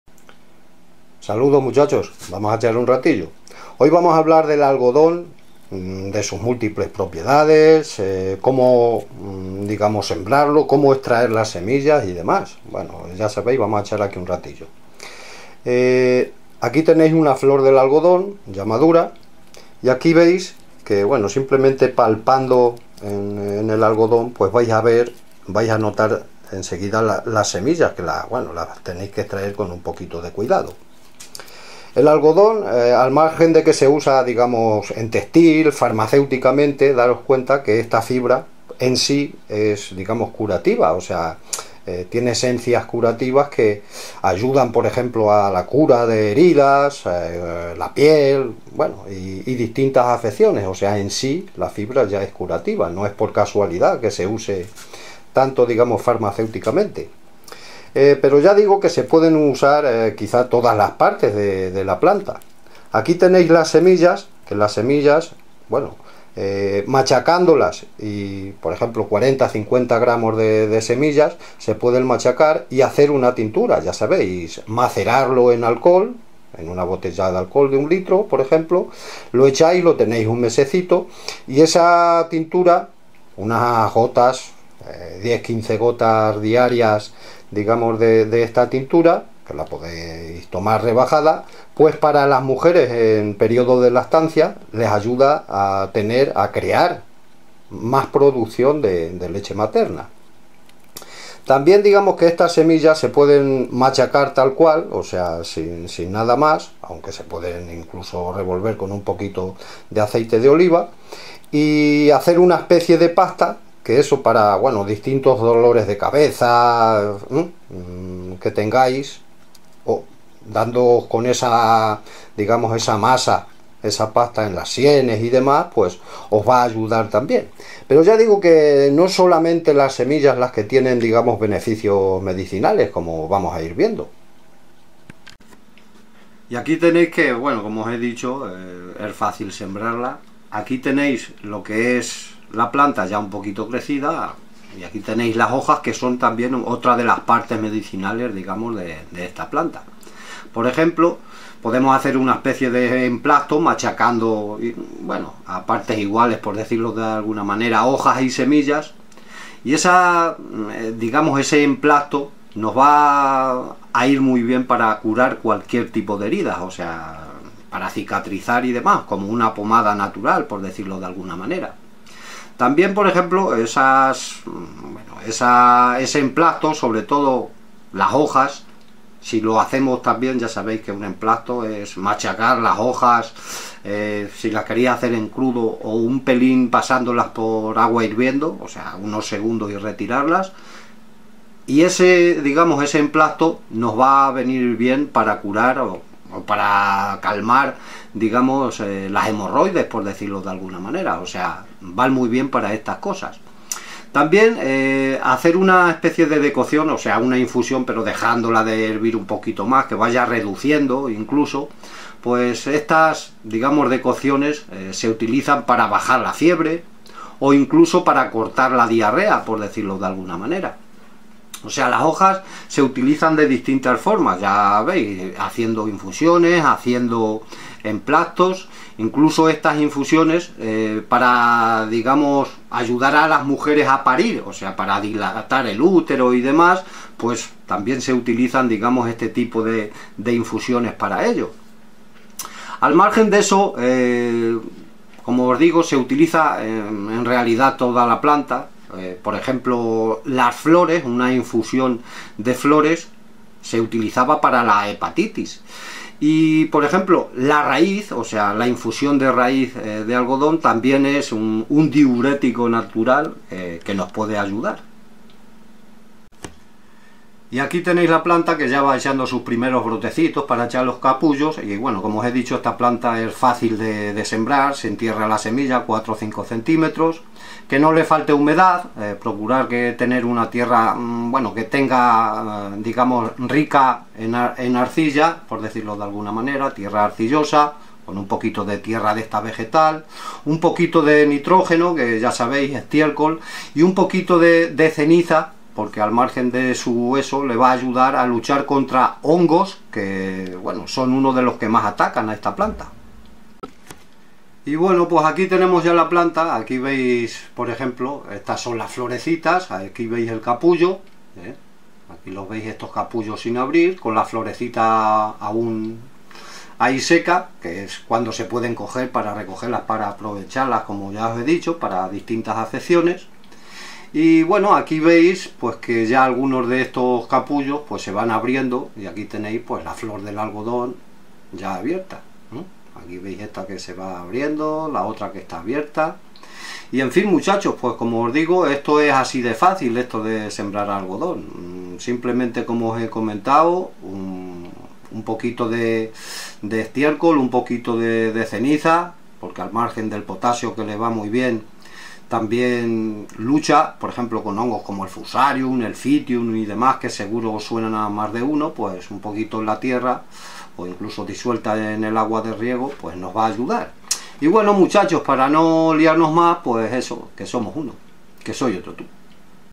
Como y porque sembrar algodón y como obtener su semilla.
Veremos como obtener semilla de la planta del algodón, como sembrarlo y daremos un repaso a sus propiedades y utilidades medicinales.